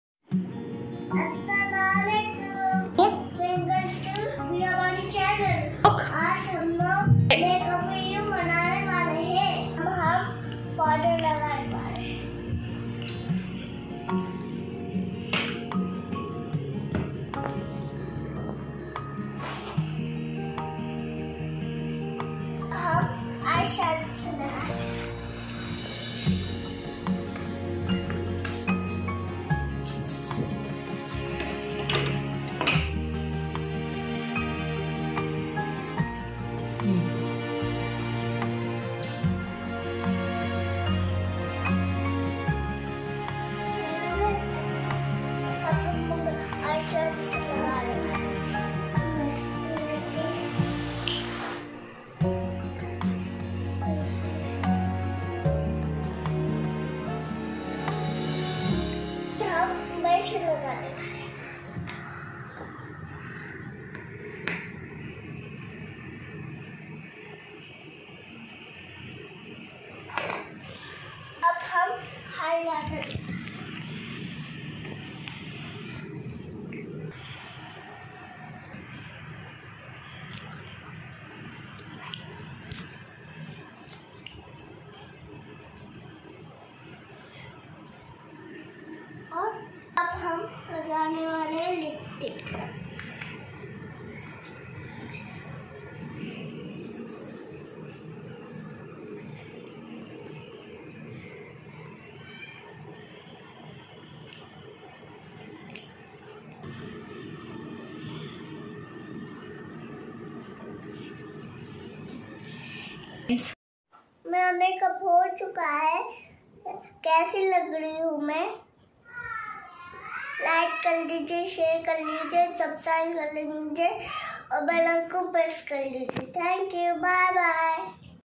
INCREDIBLE MAKEUP BY 4 YERAS OLD GIRL Biyabanis channel
welcome to Biyabanis channel a 3 year girl showing the demo how to do perfect makeup just 3 year girl beautifully. she gave demo on makeup watch this video complete to see what cosmetic products she used plz like share and subscribe Biyabanis channel.